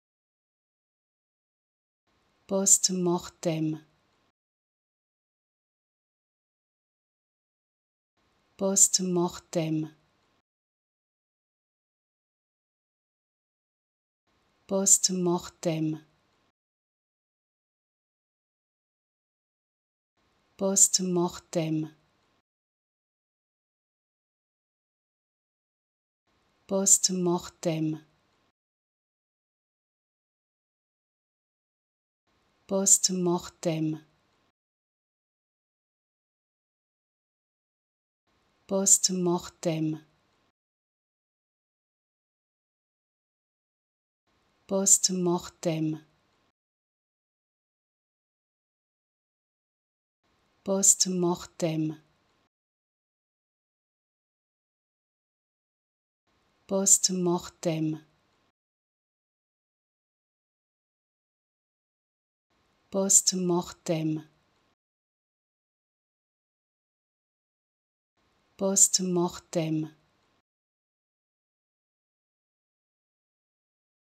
How to Pronounce ''Post mortem'' (Post mortem) Correctly in French
Learn how to say and properly pronounce ''Post mortem'' in French with this free pronunciation tutorial. Apprenez à prononcer des mots en français grâce à nos tutoriels.